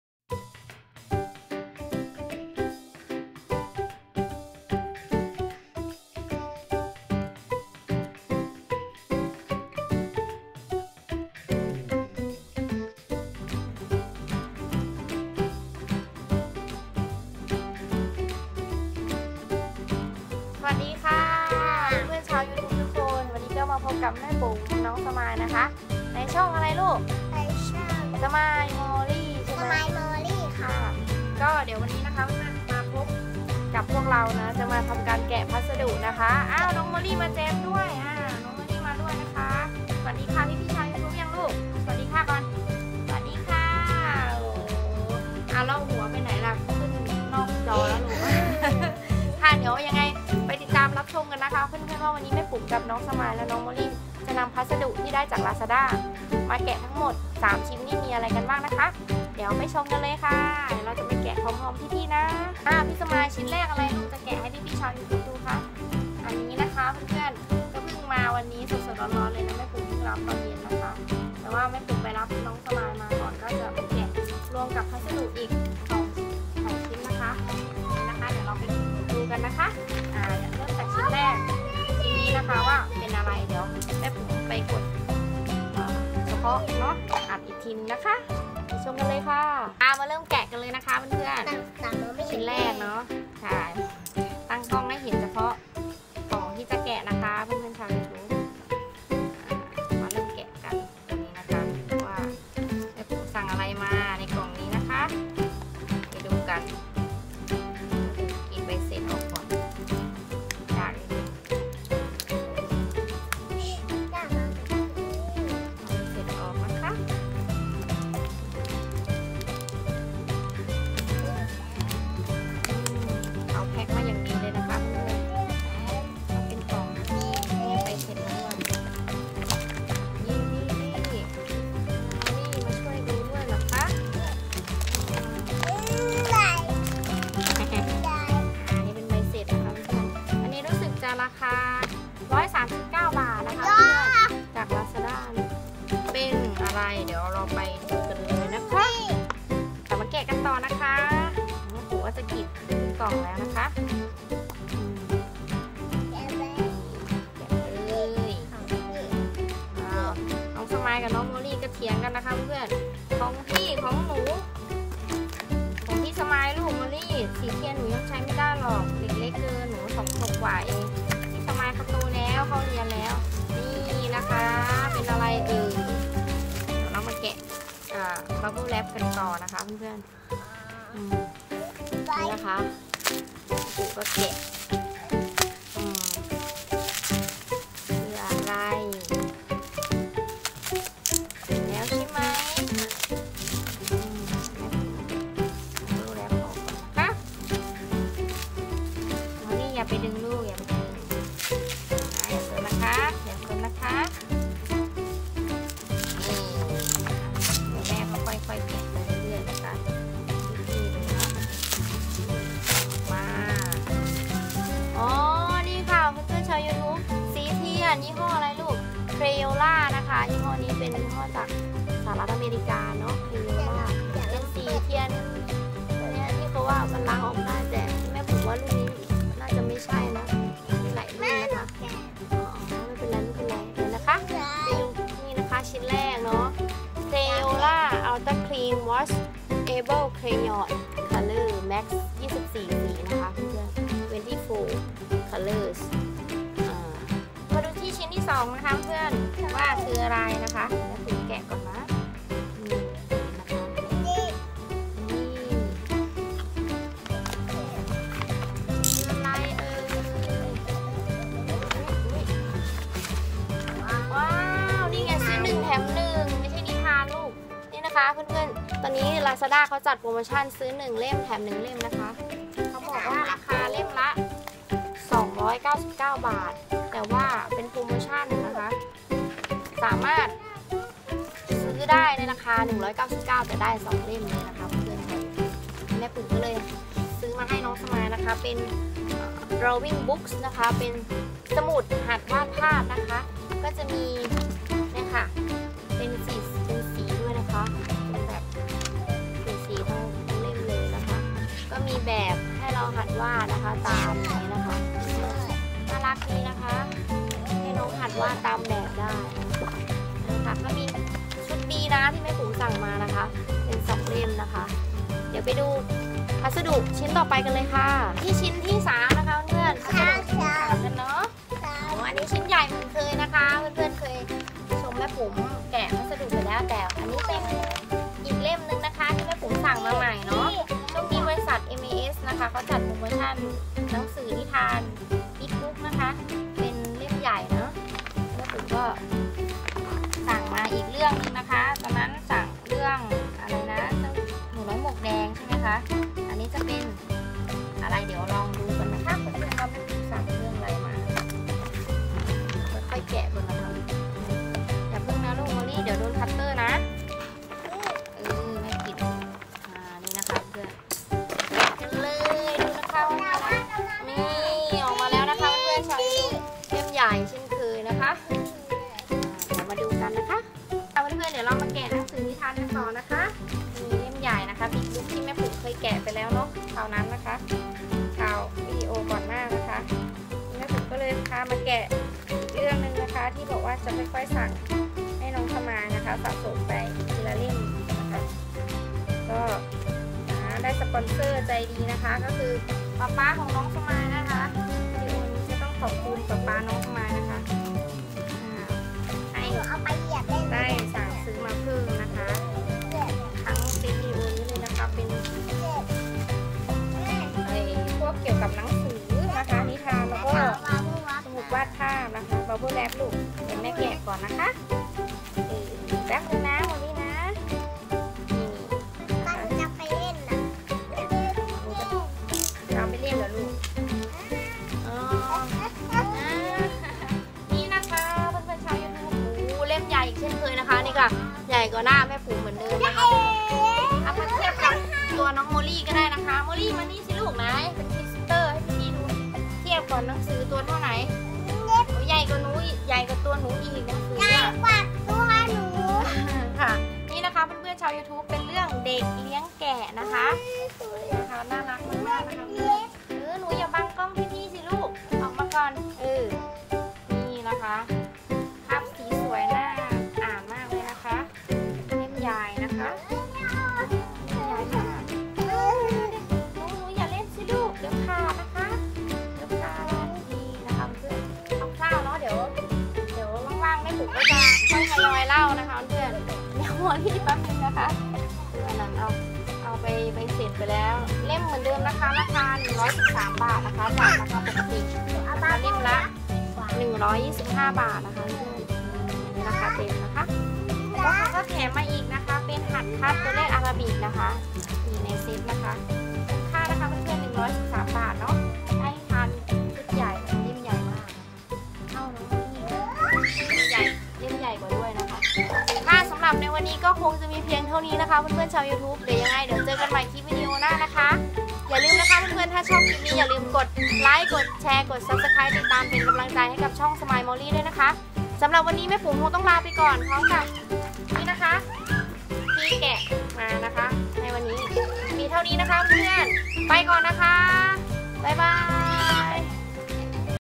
Haul รีวิวแกะพัสดุใหม่ 3 ชิ้นจากลาซาด้าค่ะ|Smile Morely
วันนี้ เป็นวันที่แม่ปุ๋มรับพัสดุสินค้า ที่ได้สั่งกับลาซาด้า ขาประจำเช่นเคยค่ะ ได้มีโอกาสนำมารีวิวแกะพัสดุใหม่ให้เพื่อนๆชาวยูทูป ได้ชมไปพร้อมๆกันค่า ต้องขอบคุณปาป๊าหมีของน้องสมายล์มอร์ลี่ ที่เป็นสปอนเซอร์ ใจดีอีกเช่นเคยจ้า สนับสนุนสินค้าทั้งหมดในวีดีโอนี้ค่ะ ในวีดีโอนี้ จะมี3รายการ ค่ะ ชิ้นแรก สีเทียน 24 สี 😊Crayola ชิ้นที่สองเป็นหนังสือสอนวาดภาพ ซื้อ1แถม1 ค่ะ  ชิ้นที่สาม เป็นหนังสือนิทานเล่มใหญ่ big book premium edition จากสำนักพิมพ์ mis ค่า                             ขอขอบคุณเพื่อนๆ ทุกคนที่แวะมาติดตามชมให้กำลังใจกันมาโดยตลอดนะคะ และเพื่อนใหม่ที่พึ่งเข้ามาแม่ปุ๋มฝาก กดติดตาม กดไลค์ กดแชร์ เป็นกำลังใจให้ช่องน้อง Smile Morely กันด้วยนะคะ🙏🤗😍💖